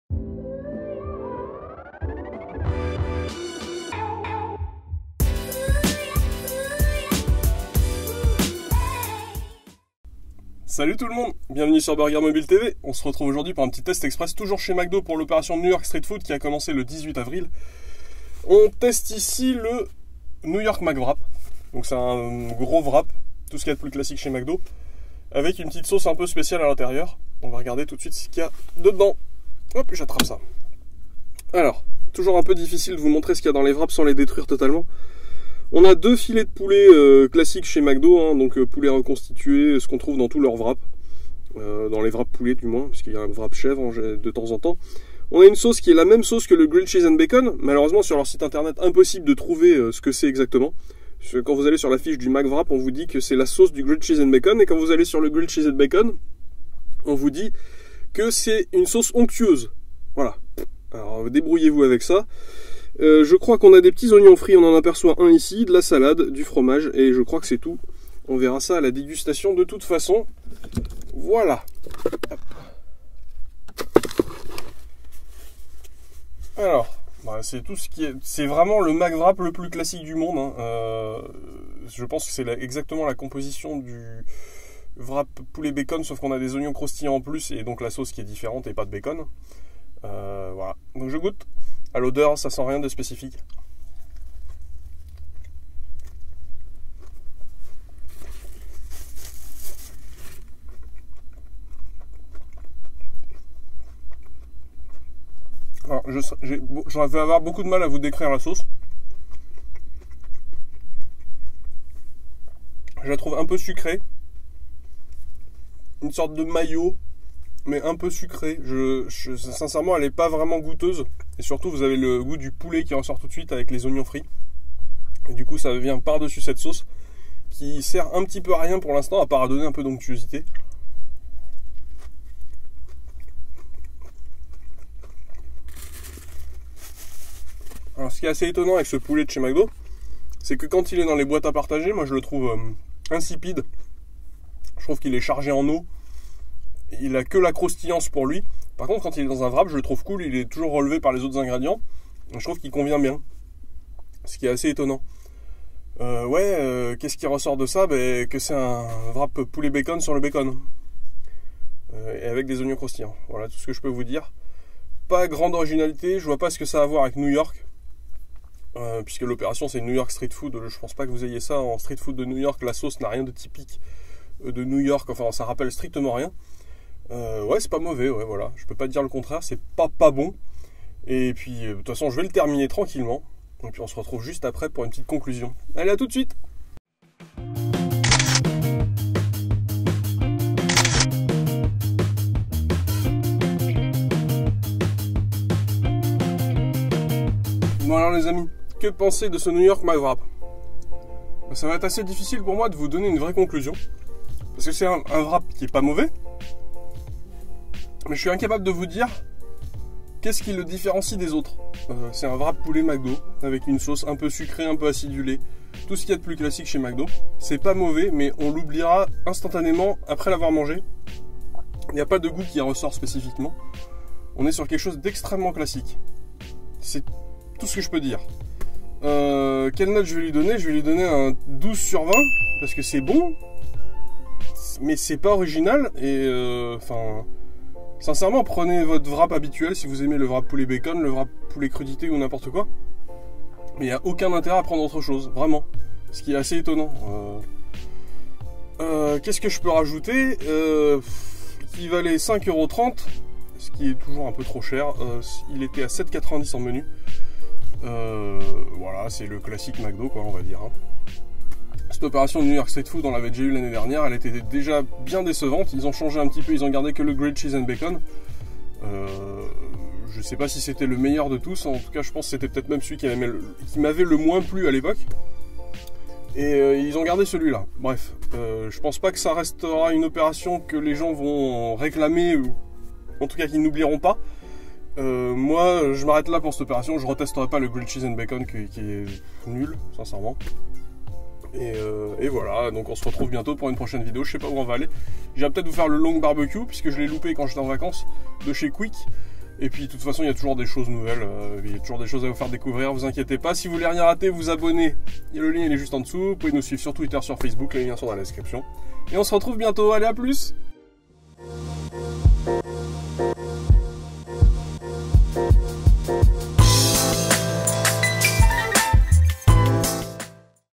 New York McWrap - McDonald's - Test Express
On se retrouve pour un Test Express !
On teste ici le New York McWrap, sorti en début de semaine pour l'opération New York Street Food, chez McDo. Un Wrap poulet très très classique avec une sauce mystére...